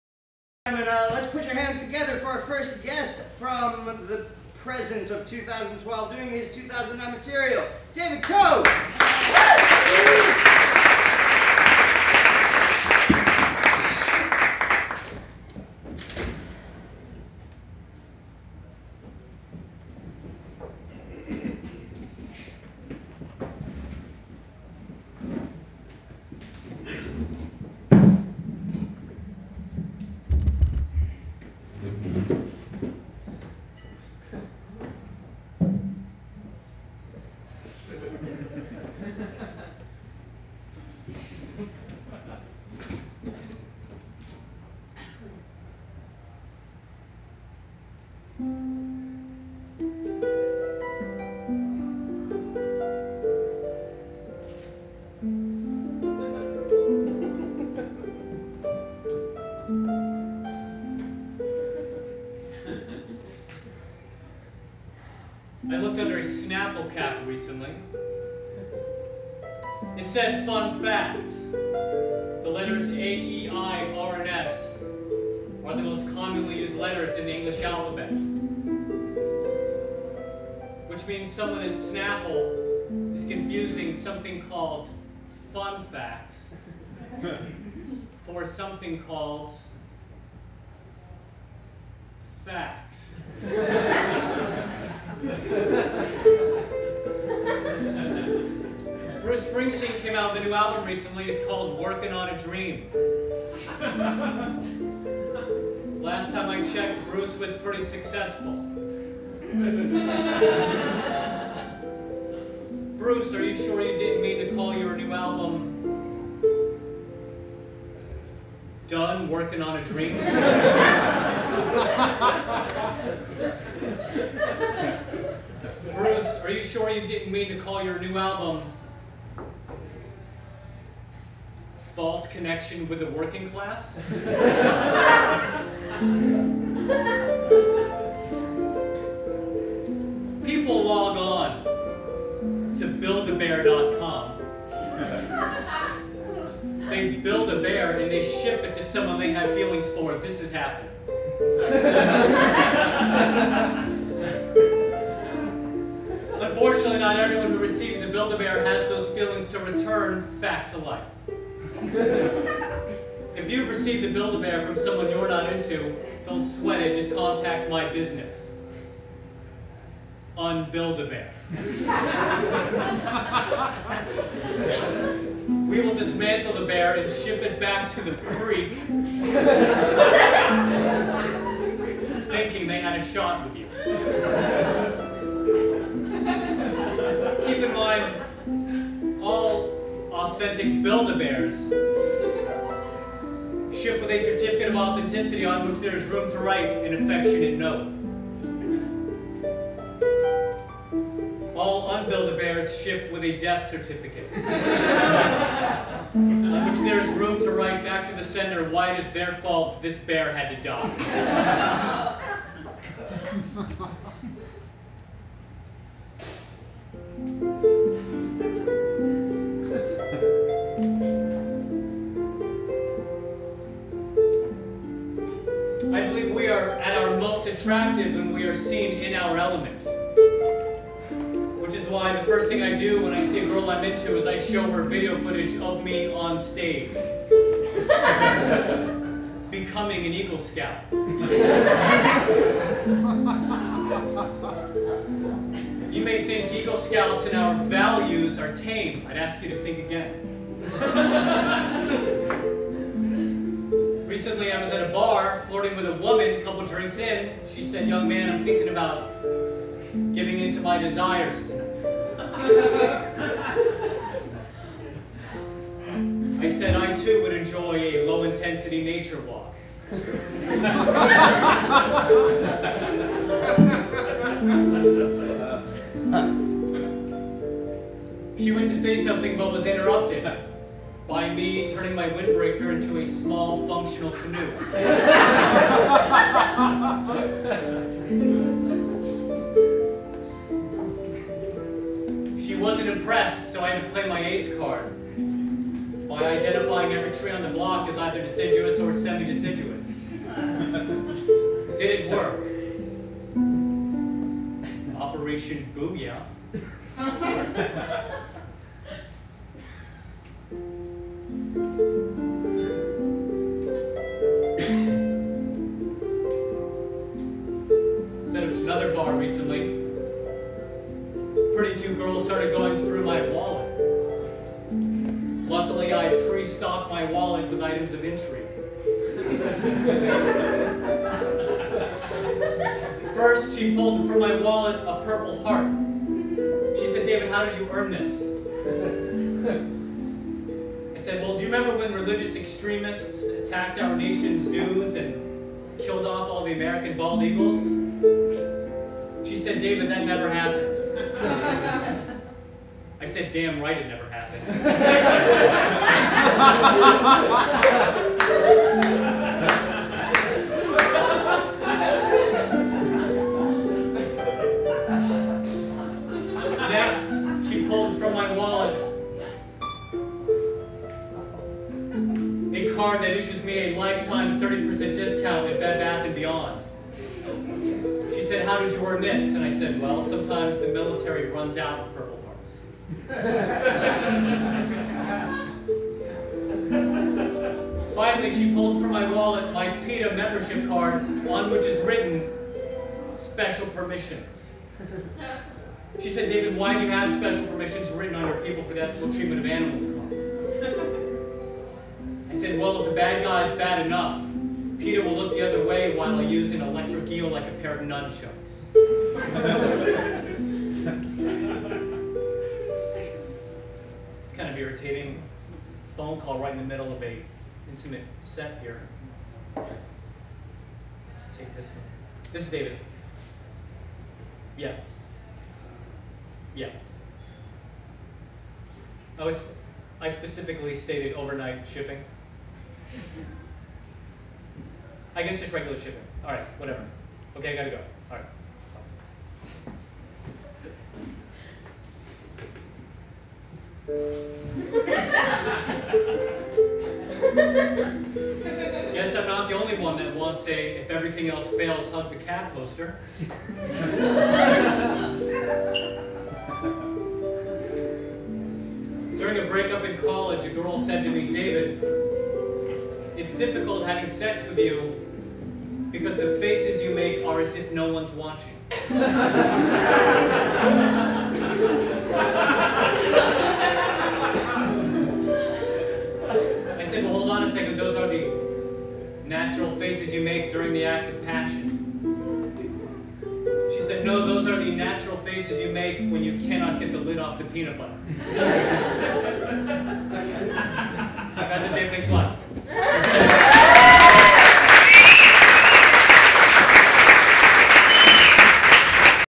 David Cope and His Harp: Live on The Moon
David Copes strums our heartstrings on the 19th Moon on September 22, 2009.

Shot by: Tim Whitney

TheMoonShow.com